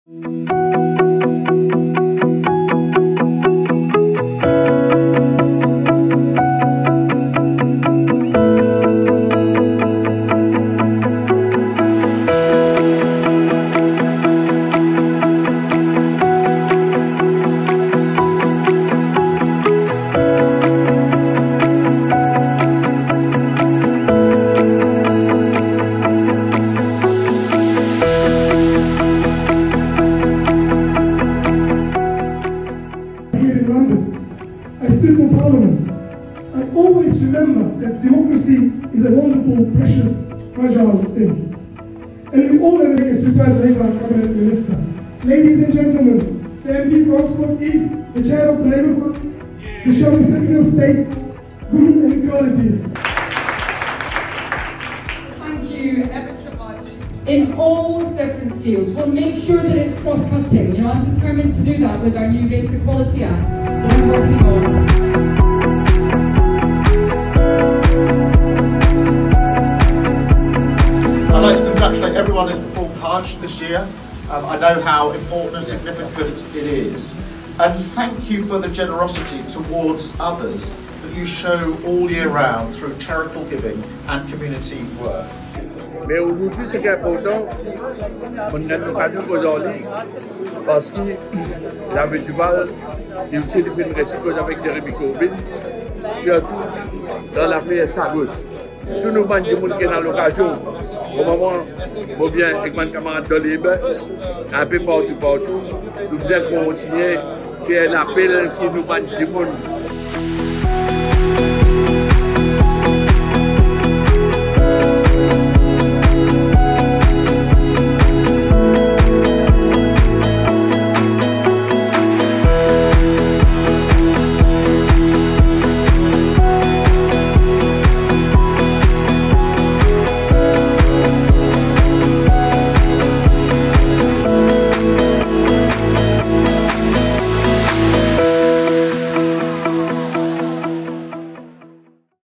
Media Reception organised by the Labour Party (04.09.2023)
General election in Britain coming next year. Labour Party realising the importance of BAME media. So there engagent has started! Today SME4 Labour invited media to interact with some MPs and members of the party. Chairman Anneliese Dods, David Lamy Shadow foreign secretary and Ibrahim Dogus were speaking to the media about importance of engagement.

Excellent Connecting Communities Media Reception organised by the Labour Party to thank the BAME media for their brilliant contribution in the UK. Photo: Chairman of British Muslim Friends of Labour, Shokat Ali, Anneliese Dodds MP, David Lammy MP, Ibrahim Dougus, Shahid Nadeem and Afzal Khan MP.